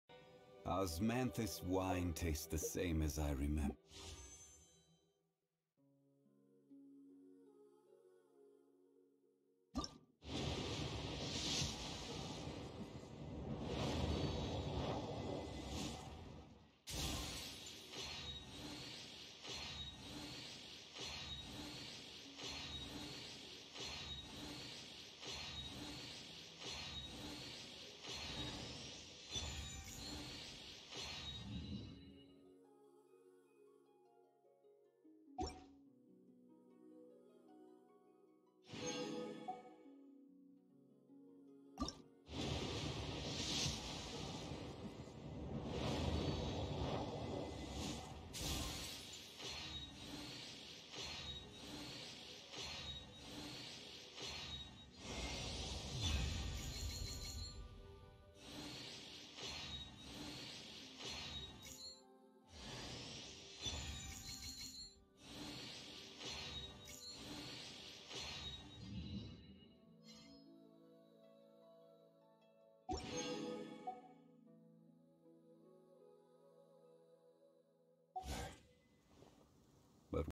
Kazuha pulls - Gone right lol
well i was saving for ayaka but i could not control the urge  and was at 50/50
and won so sry ayaka